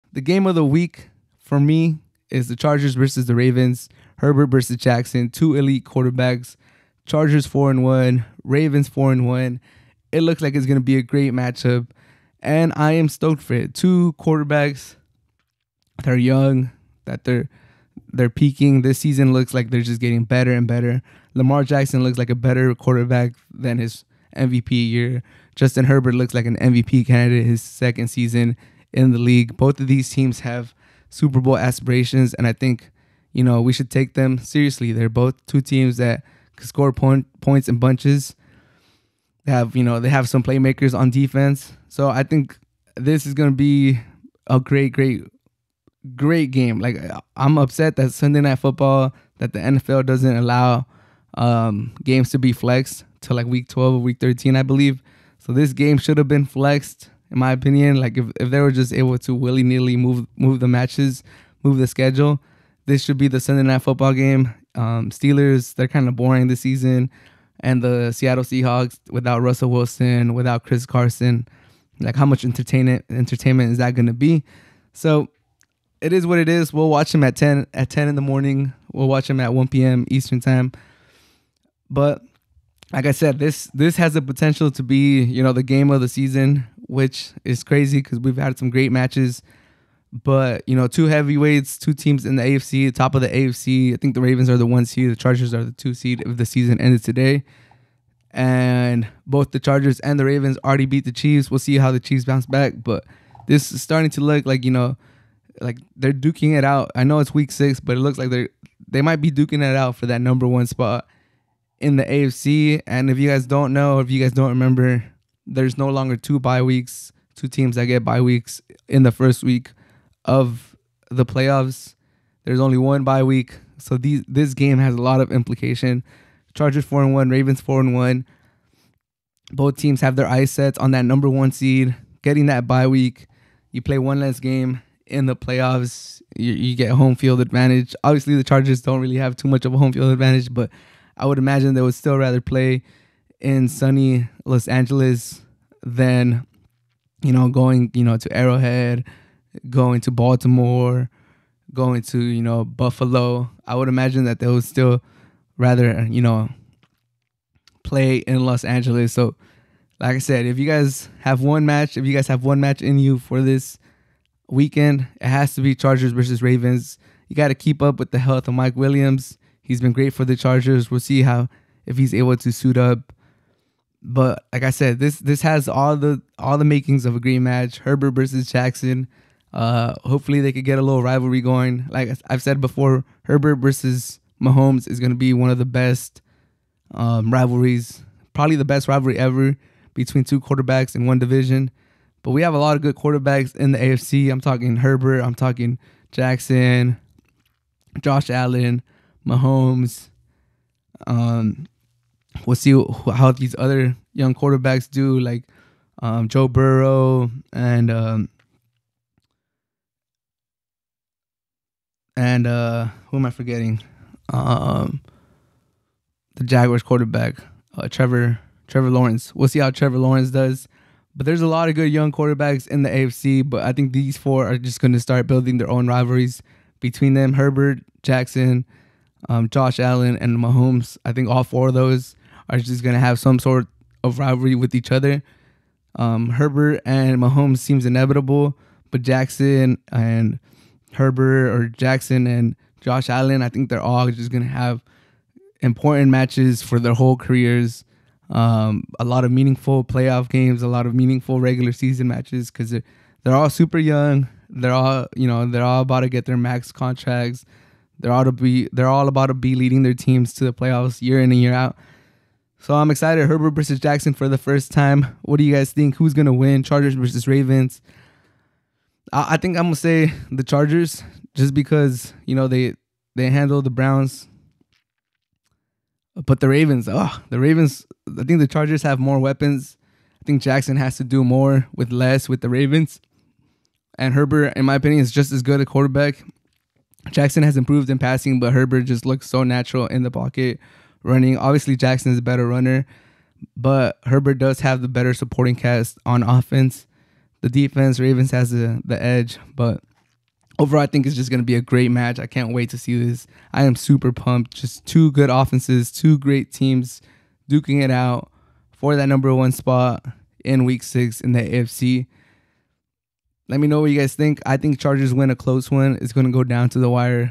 Lamar Jackson vs Justin Herbert | Chargers vs Ravens Preview
Lamar Jackson vs Justin Herbert
Chargers vs Ravens
This is the Game of the Week in the NFL week 6. It is going to be ELECTRIC.

Amazon Music